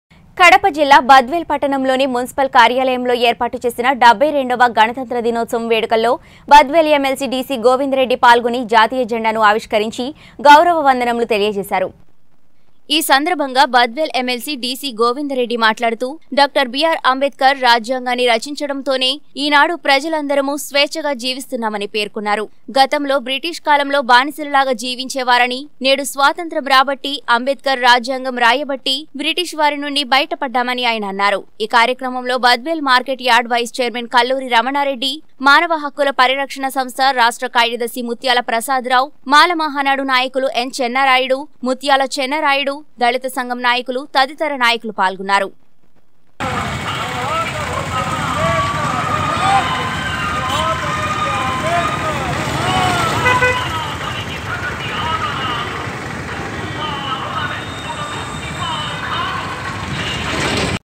బద్వేల్ పట్టణంలో రిపబ్లిక్ డే వేడుకలలో పాల్గొన్నఎమ్మెల్సీ గోవిందరెడ్డి - కడప జిల్లా
బద్వేల్ పట్టణంలో రిపబ్లిక్ డే వేడుకలలో పాల్గొన్నఎమ్మెల్సీ గోవిందరెడ్డి - కడప జిల్లా  ||19News Telugu